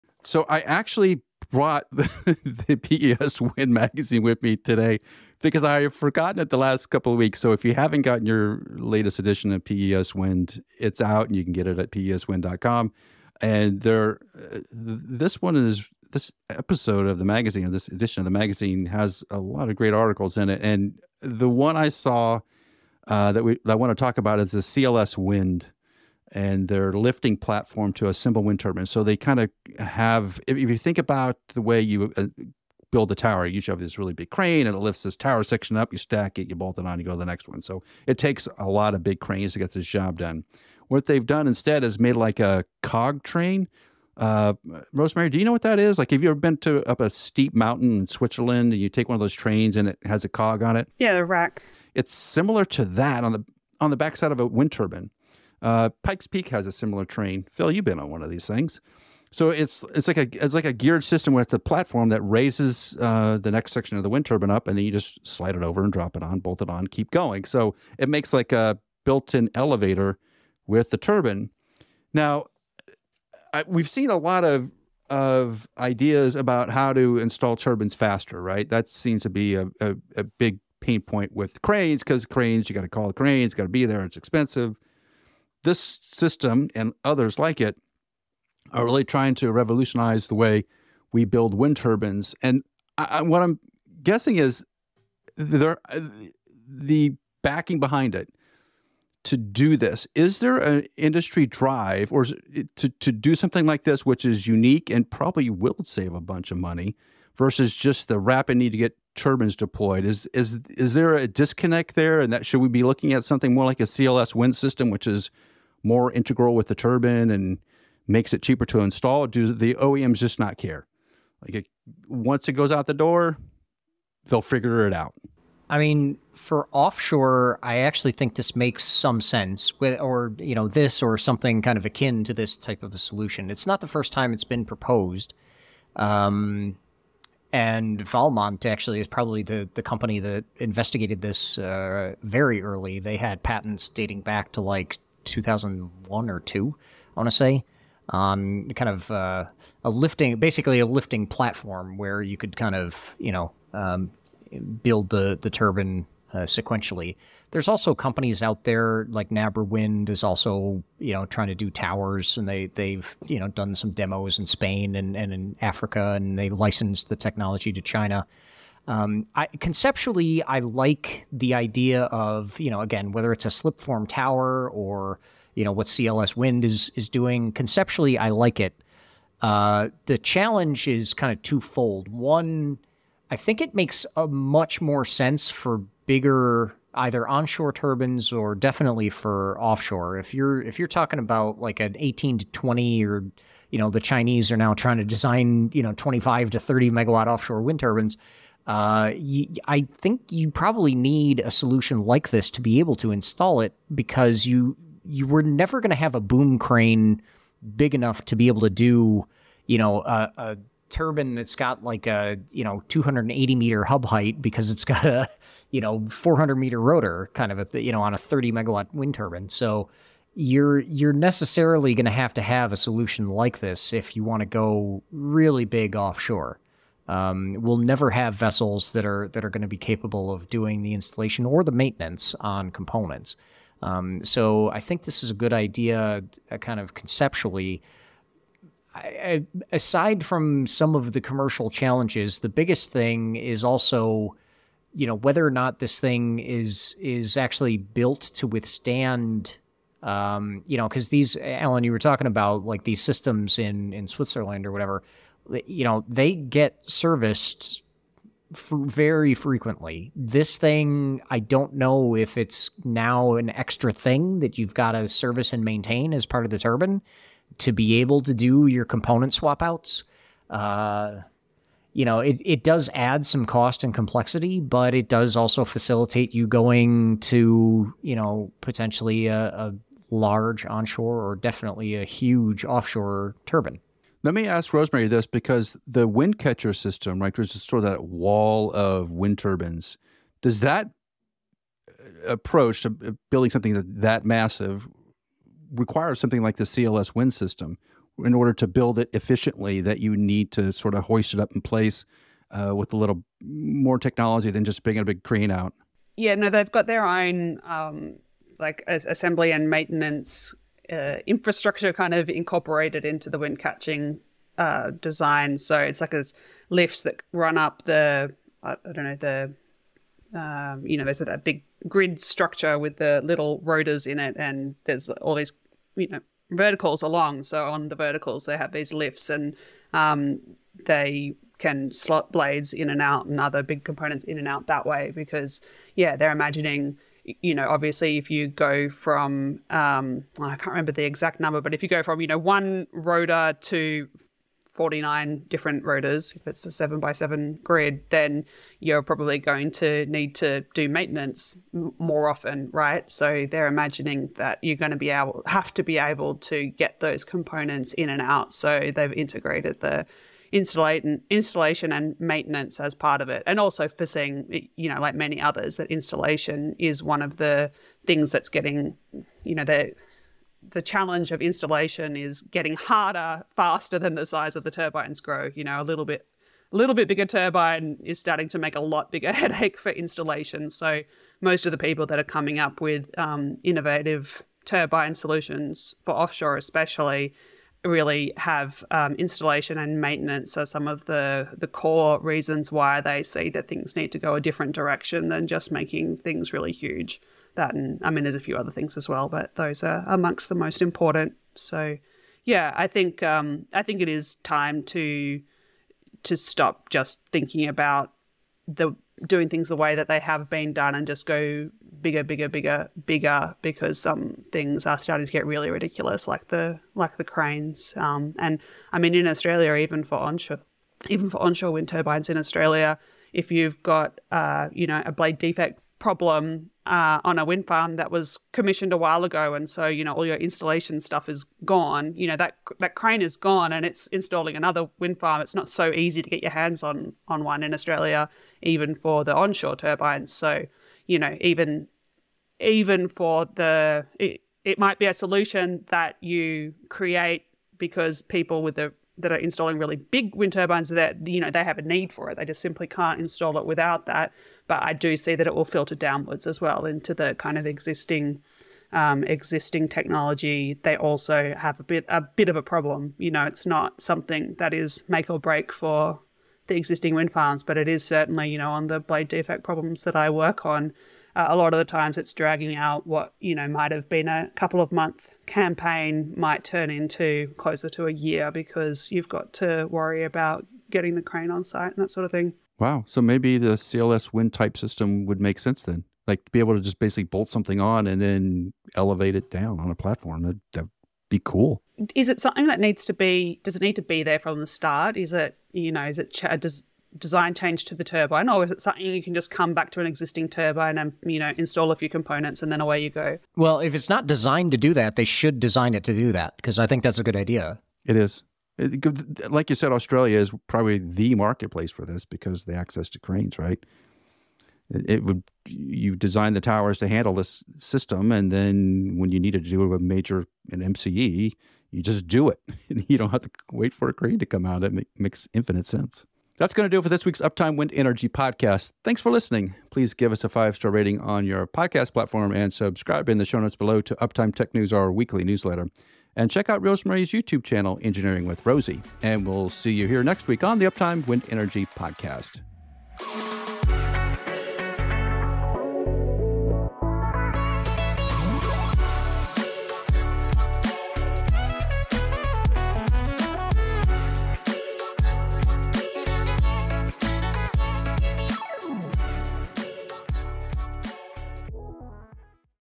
CLS Wind Innovates Turbine Assembly
Allen, Phil, and Rosemary discuss an article from the latest edition of PES Wind Magazine from CLS Wind about their lifting platform used to assemble wind turbines. Read the article

⚡⚡CONNECT WITH WEATHER GUARD LIGHTNING TECH